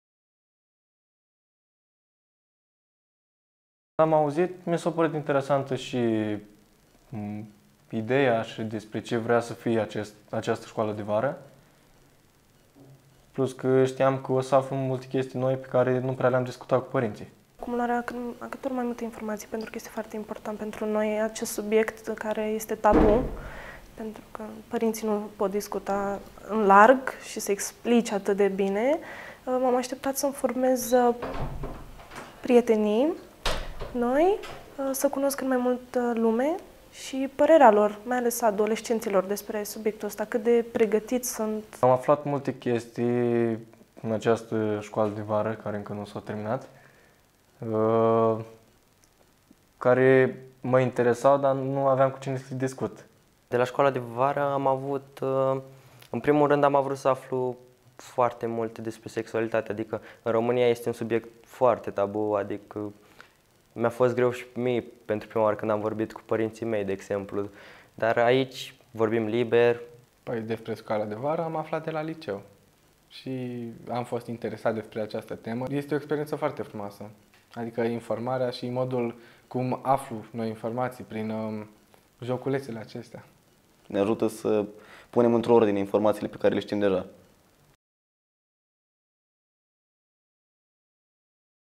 ȘCOALA DE VARĂ “BE SMART! BE SAFE!”
Pentru a veni în sprijinul adolescenților, fete și băieți, cu vârste între 10 și 18 ani, UNICEF a dezvoltat un model de intervenție integrată, numit Oportunitati pentru toti adolescentii, pe care îl va implementa pe o perioadă de trei ani în cinci orașe: București, Cluj-Napoca, Iași, Constanța și Bacău.
Modelul a fost dezvoltat plecând de la analiza determinanților ce influențează viața adolescenților din România și a serviciilor existente pentru aceștia la nivel local, analize derulate în parteneriat cu Ministerul Tineretului și Sportului.
Parte din model a fost inclusa si organizarea unei scoala de vara “BE SMART! BE SAFE!”  de catre Fundatia Alaturi de Voi (AVD) cu adolescenti din Iasi si Constanta la Moieciu in perioada 23-26 iulie 2015.